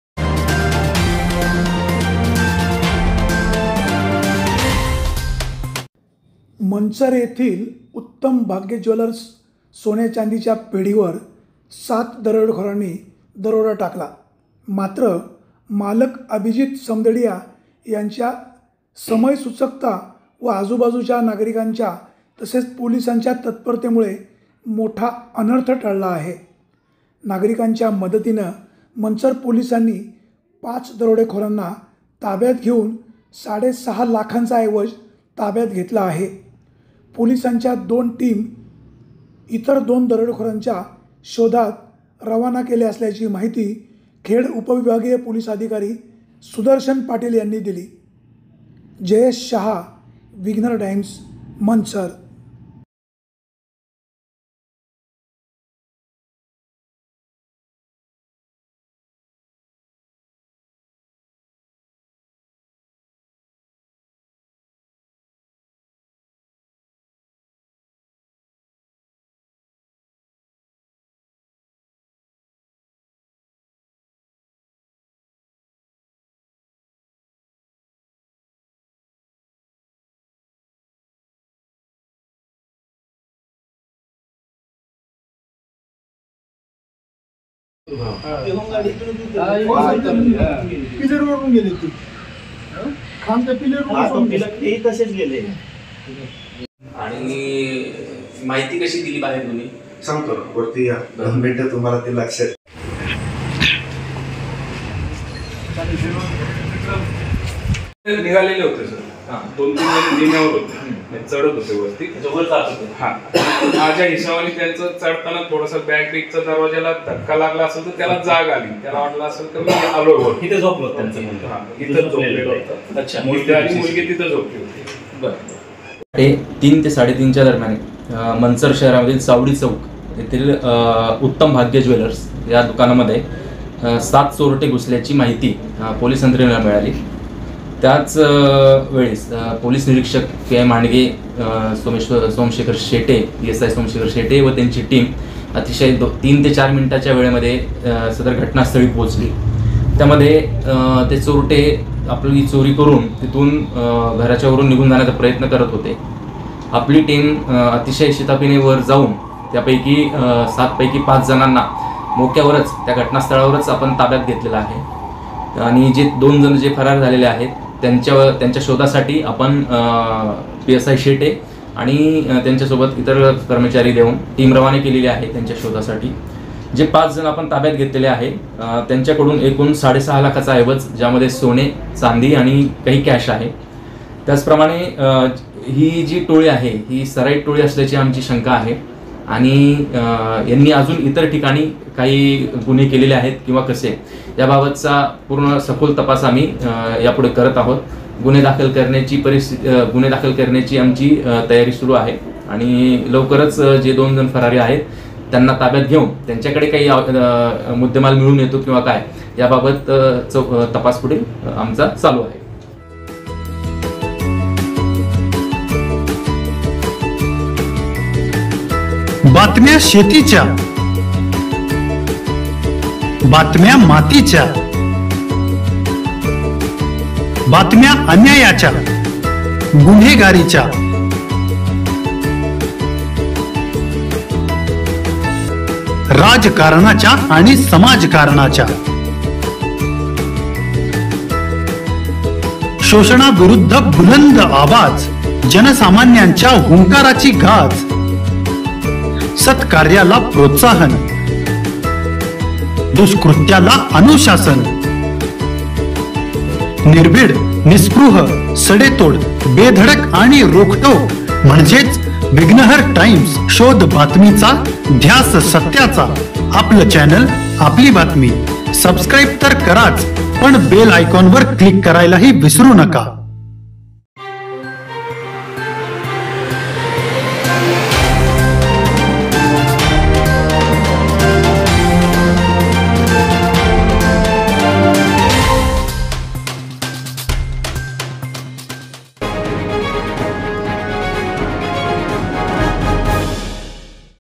मंचरच्या उत्तम भाग्य ज्वेलर्स या सोन्या-चांदीच्या पेढीवर सात दरोडेखोरांनी दरोडा l मालकाची सतर्कता l
मंचर :  टाकला मात्र मालक अभिजित समदडीया यांच्या समयसूचकता व आजूबाजूच्या नागरिकांच्या तसेच पोलिसांच्या तत्परतेमुळे  मोठा अनर्थ टळला आहे, नागरिकांच्या मदतीने मंचर पोलिसांनी पाच दरोडेखोरांना ताब्यात घेऊन साडेसहा लाखाचा ऐवज ताब्यात घेतला आहे . पोलिसांच्या दोन टीम इतर दोन दरोडेखोरांच्या शोधात रवाना केले असल्याची माहिती खेड पोलीस उपविभागीय अधिकारी सुदर्शन पाटील यांनी दिली.